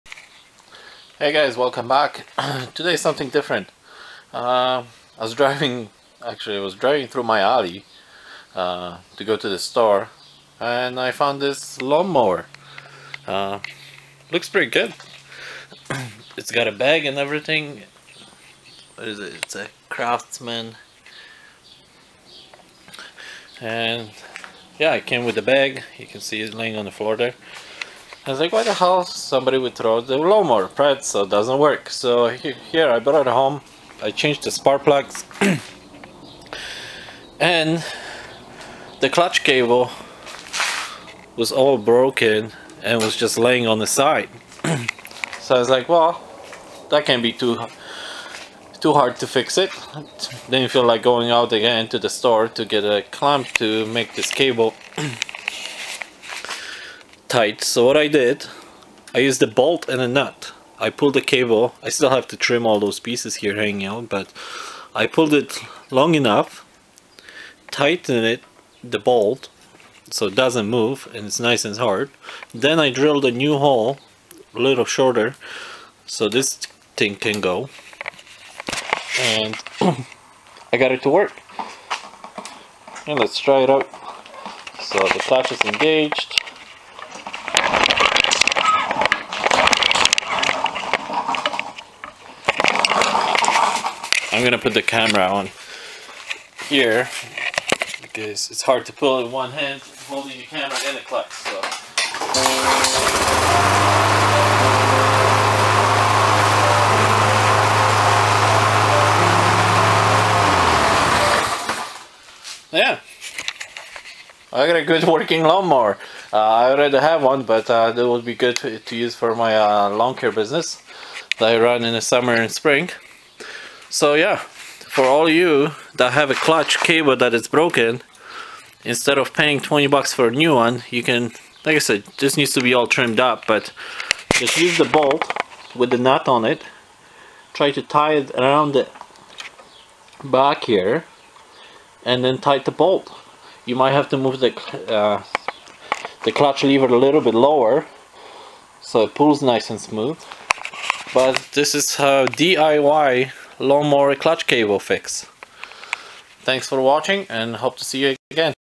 DIY Lawn mower repair
DIY clutch cable repair on Lawn Mower